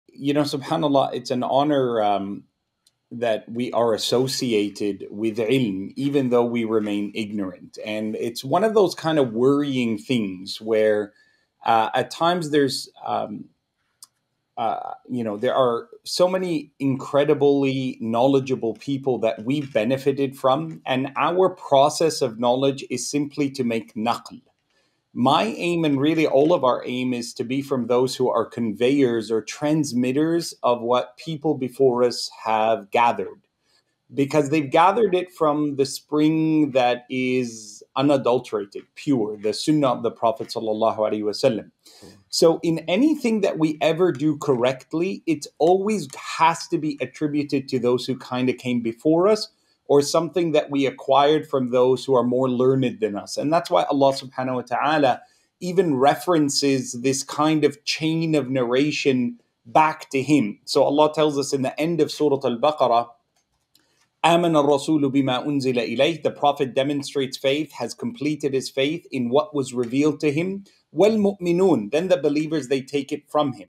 Attributing Our Blessings to Allah | Sincerely, Yahya Ibrahim
It is the Sunnah of our Prophet ﷺ to attribute our blessings to Allah. What are you grateful for today?